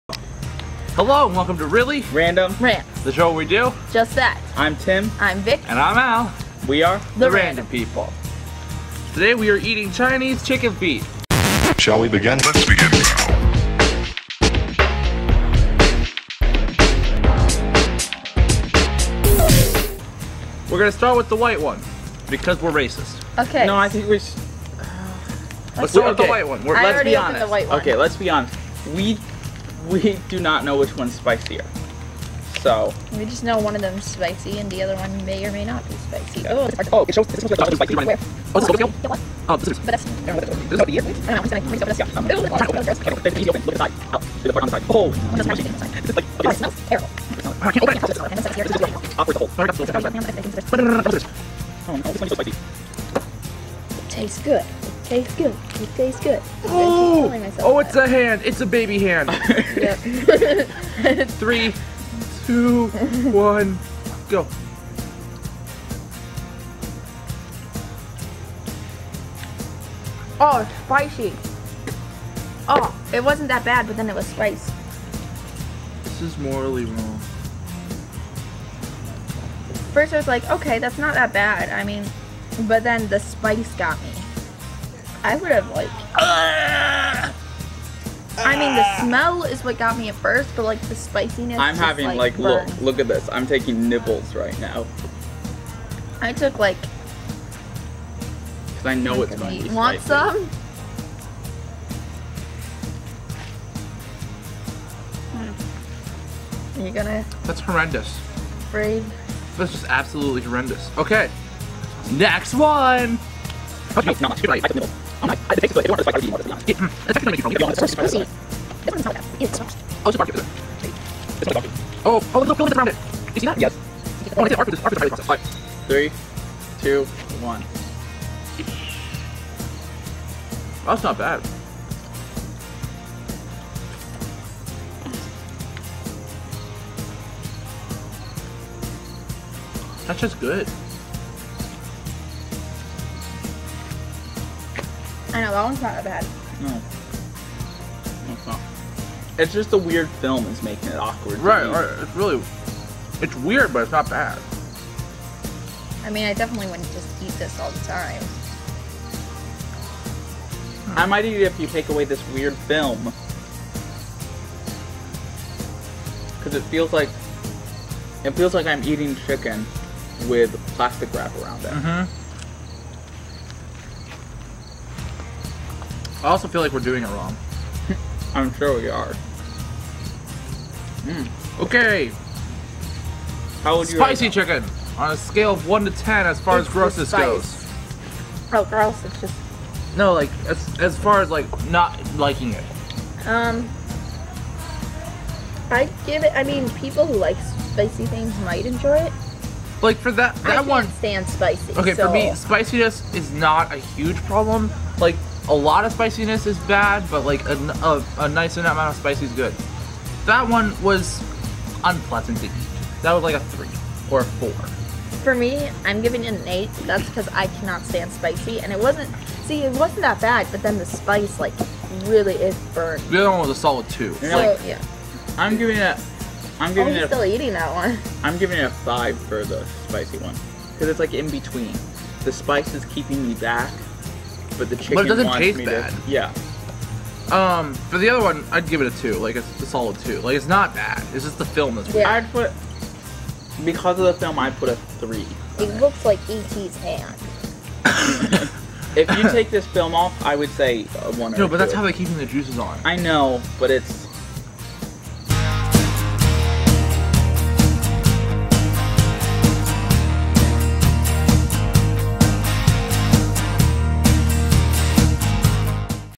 Eating Chicken Feet...From China!
Today we eat food straight from China! Not enough to convince you to watch? Well it's also Chicken Feet! That's right we're eating the feet from Chicken.
Is it good? Is it Bad? Does it taste like Chinese Chicken?
Find out today...